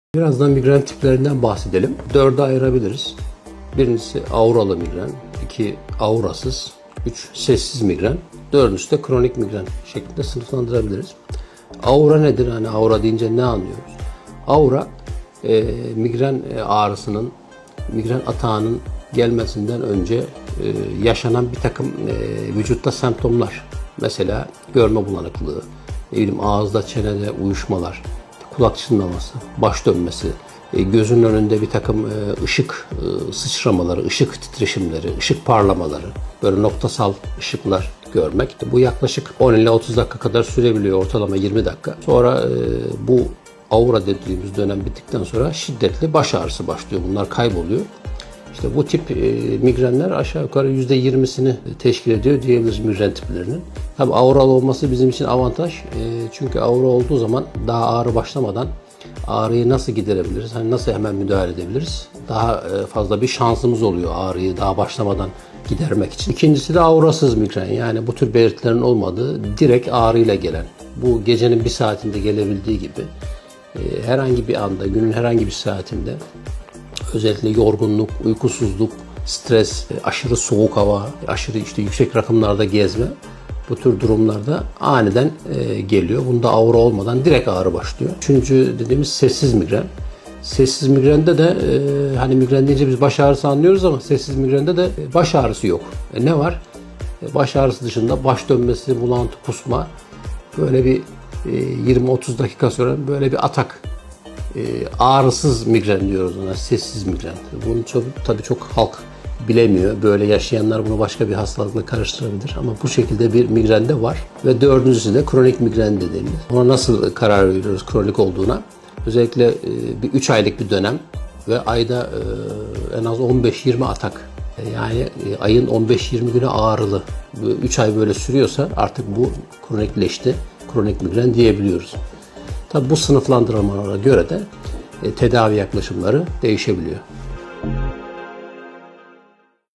Migren Türleri Nelerdir?
Nöroloji Uzmanı Uzm. Dr. Mustafa Necdet Arı Anlatıyor.